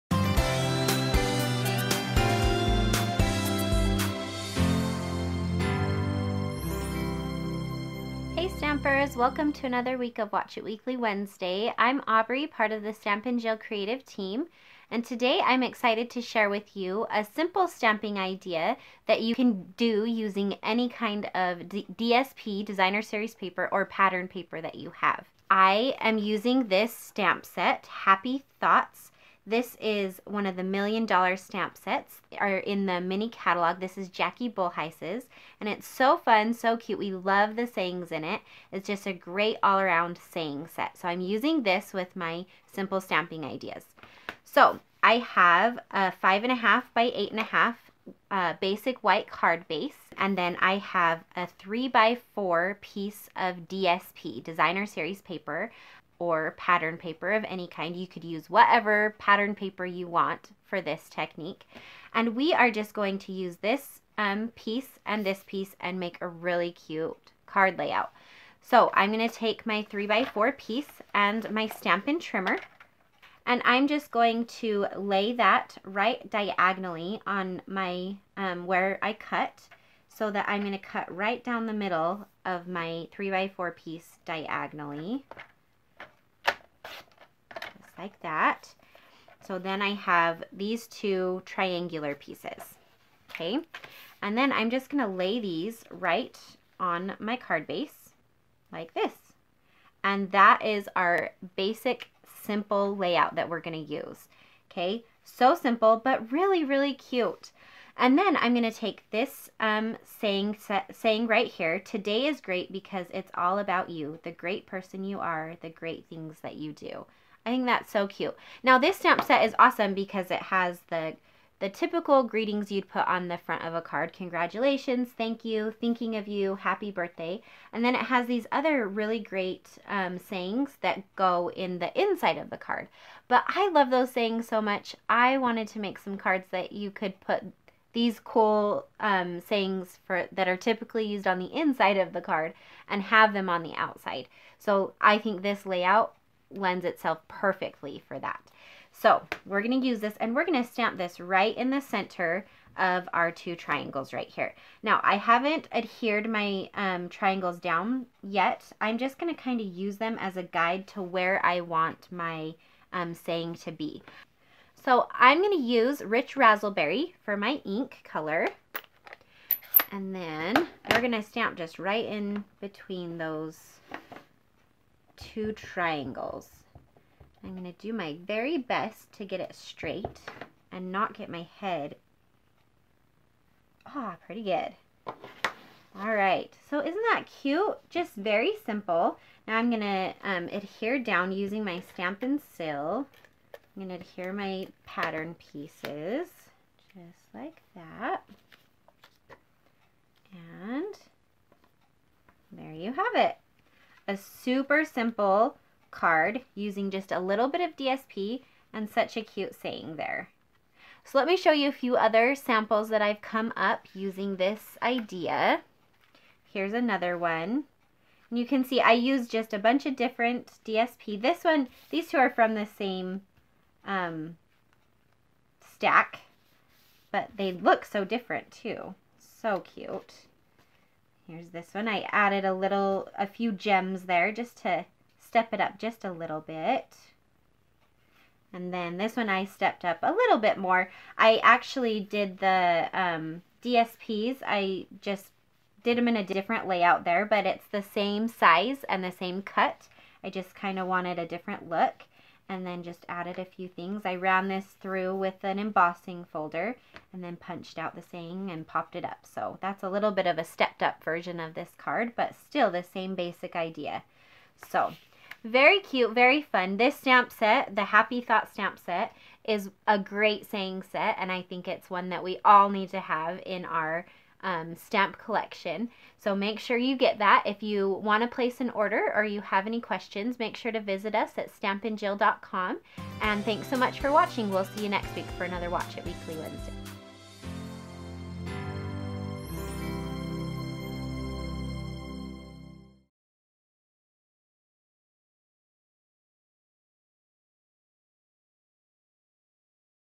Stamping Jill - Simple Layout Card Ideas
Stampin' Up! Demonstrator Aubrey demonstrates the beautiful Happy Thoughts Cling Stamp Set. Jackie Bolhuis created this stamp set and we are so excited to use it. Check out how we came up with super simple and easy card ideas.

Fine Art Floral 12" X 12" (30.5 X 30.5 Cm) Designer Series Paper [154558]
All Stampin' Up! - Designer Series Paper, Patterned Paper